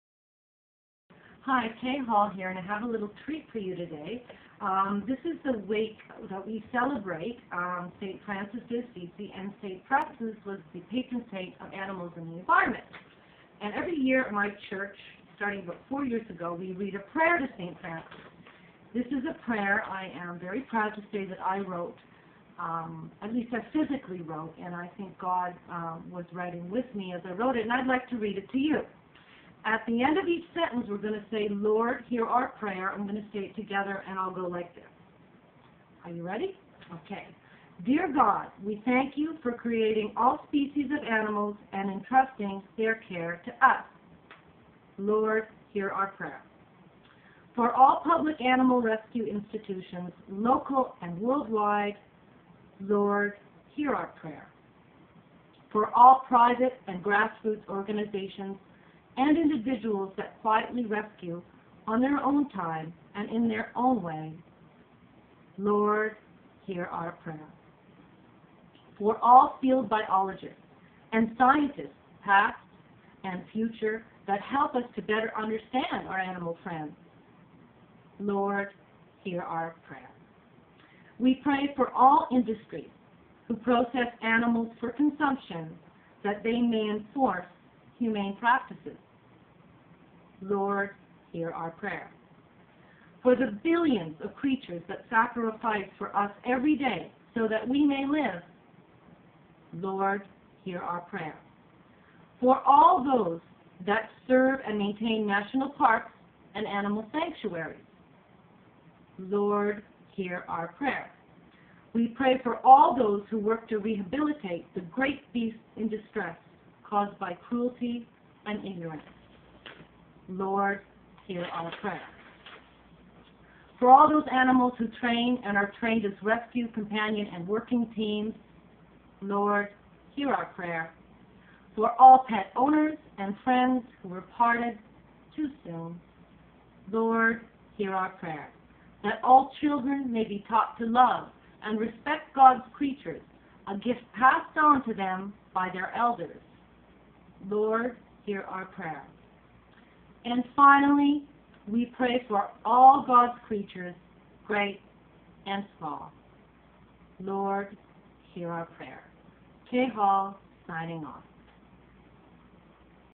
Prayer for St. Francis by Kay Hall
Kay Hall reads a prayer for St. Francis that she wrote